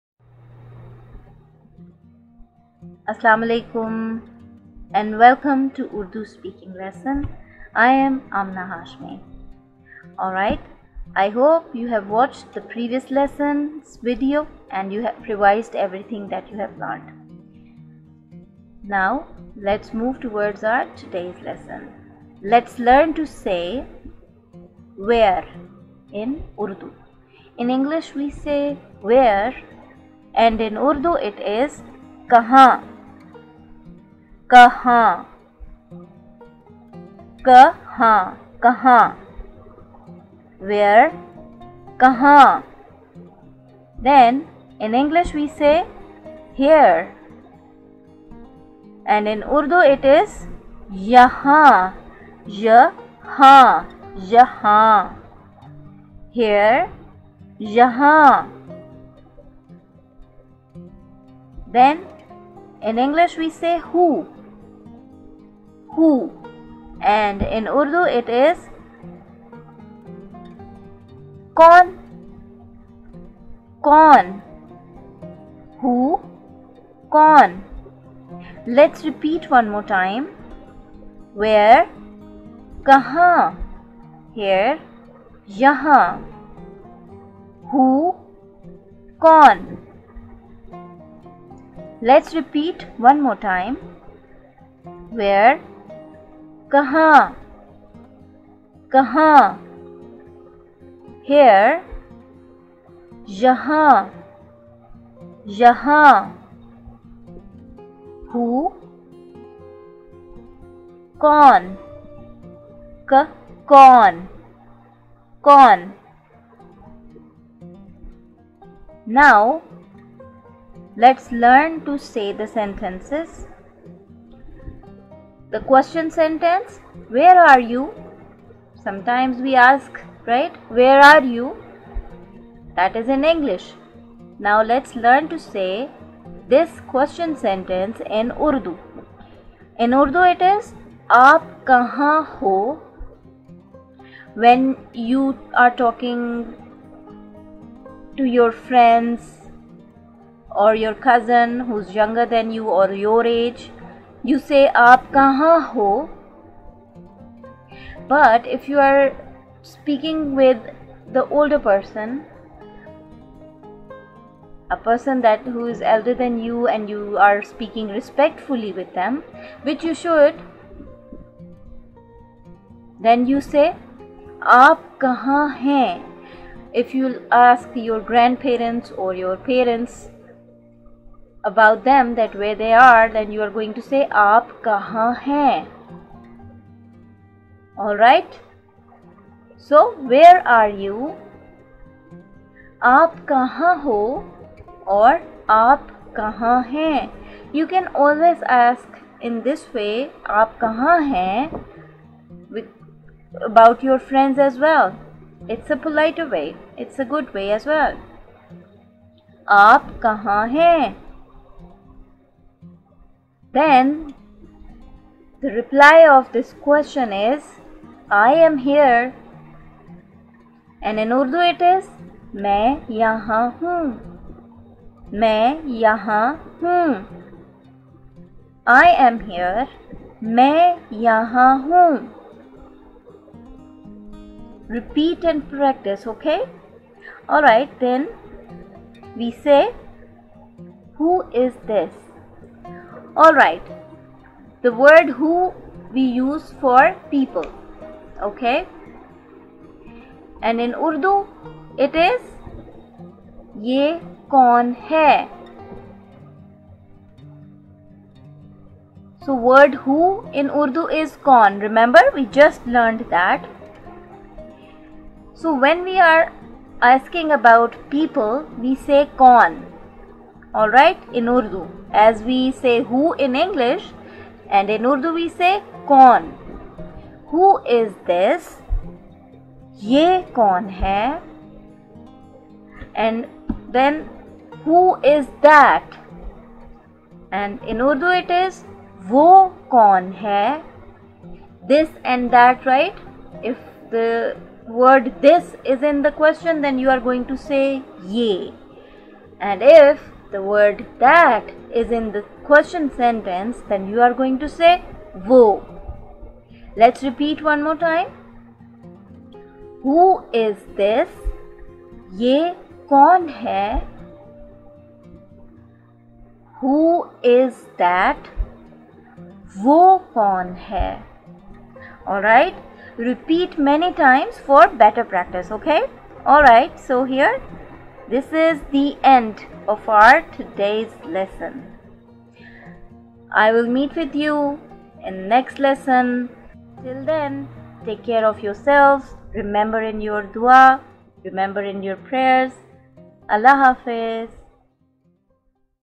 Urdu Speaking lesson 34- Learning Sentences in Urdu- (Beginners /Kids)
Hey there! This lesson is a part of the Urdu Speaking Skills.
In this video, you'll learn Sentences in Urdu and a lttle something more-